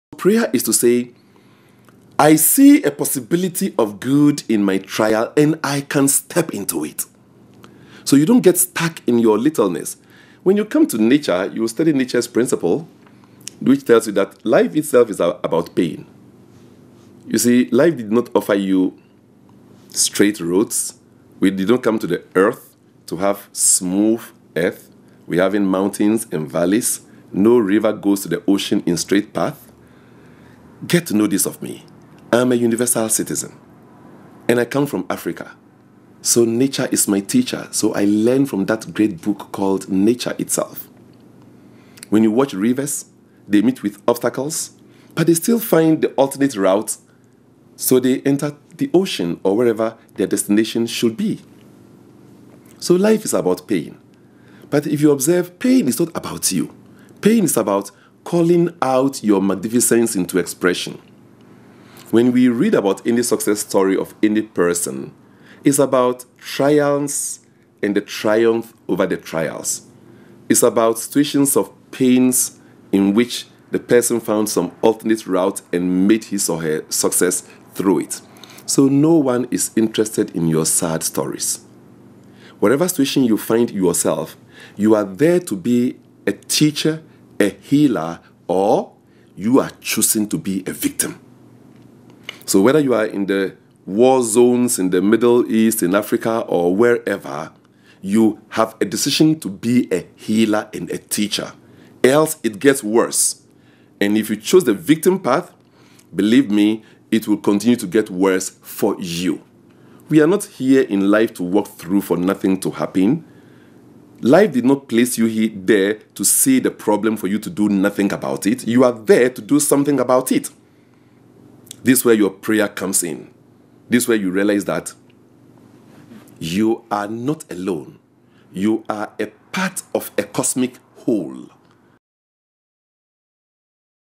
Brother Ishmael Tetteh on prayer and suffering
What is the role of prayer in turbulent times? Will we be Teachers, Healers or Victims?